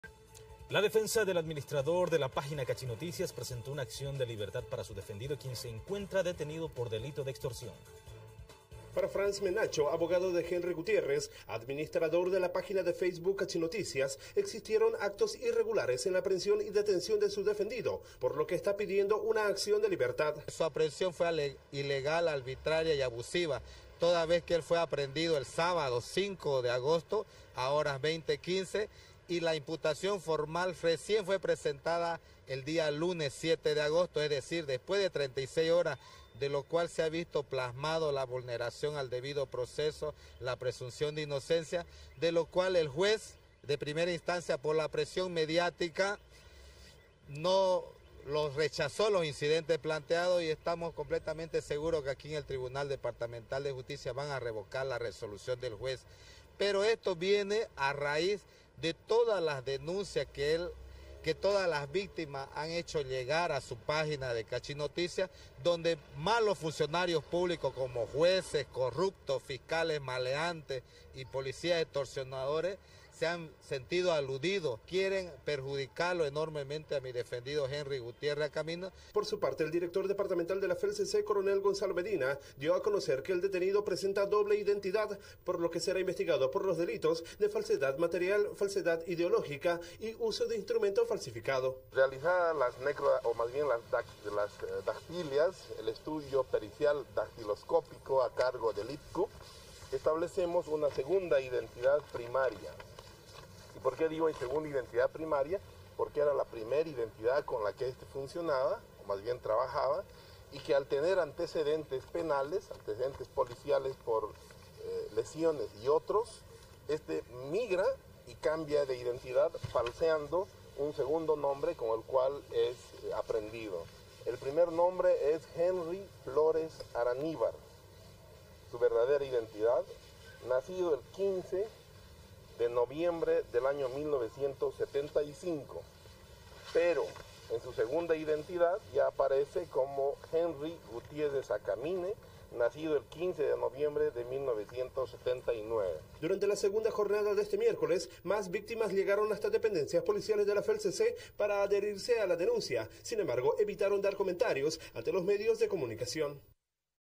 Piden libertad para administrador de "Cachinoticias"
El abogado del administrador de la página "Cachinoticias" presentó una acción de libertad para su defendido, quien se encuentro detenido por el delito de extorsión.